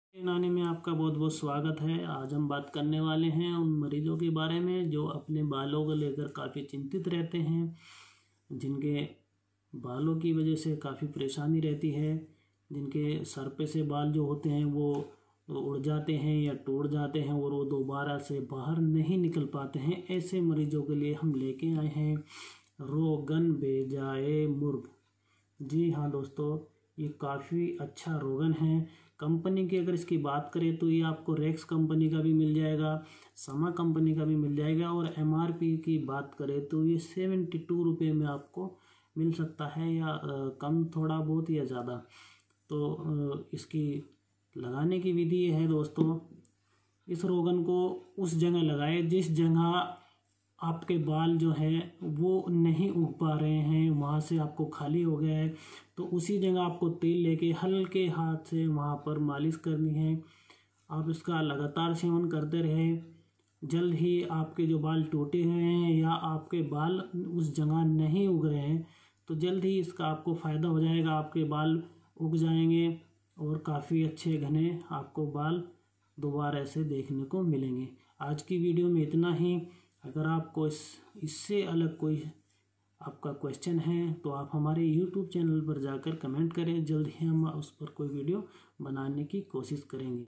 Roghan Baiza Murg Ke fayde| रोगन बैज़ामुग॔ के फाइदे | sk unani
इस रोगन के सेवन से सर के बाल दोबारा निकल जाते हैं|